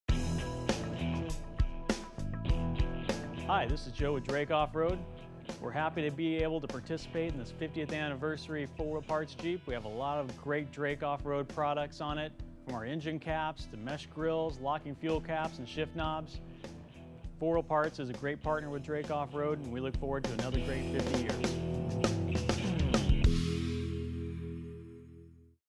Drake Off Road Wishes 4 Wheel Parts a Happy Birthday
- Johnston, Director of Sales and Marketing wishes 4 Wheel Parts a Happy Birthday. Enter for the chance to win the custom Jeep featured in the video